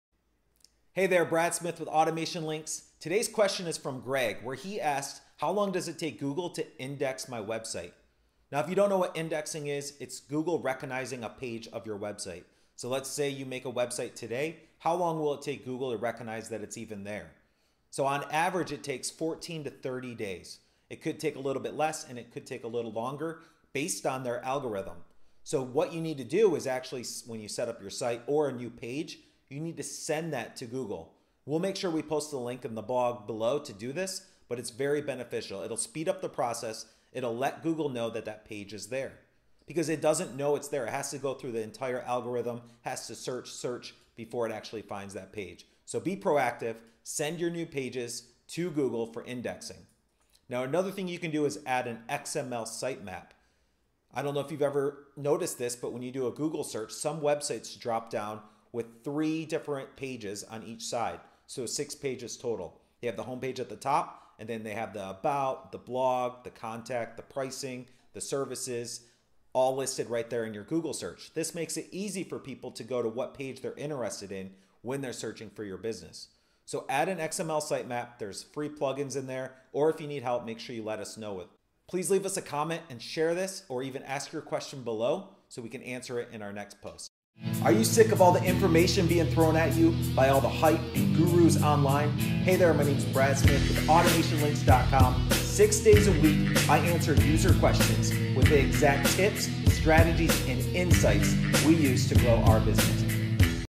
How Long Does It Take Google To Index My Website?
Index Website – In this video Brad Smith owner of AutomationLinks shares his insights on how long it takes to get your website indexed on Google. The average time you have to wait is between 4-30 days for Google to index a page on your website. If you submit your link you’re likely to get that page indexed sooner.

How to get your site indexed by Google quicker:
1. Create A Sitemap
2. Find Out If Your Site Is Indexed Already
3. Install and set up Google Analytics & Search Console
4. Use internal links on your website
5. Index your site with other search engines
6. Share your website link everywhere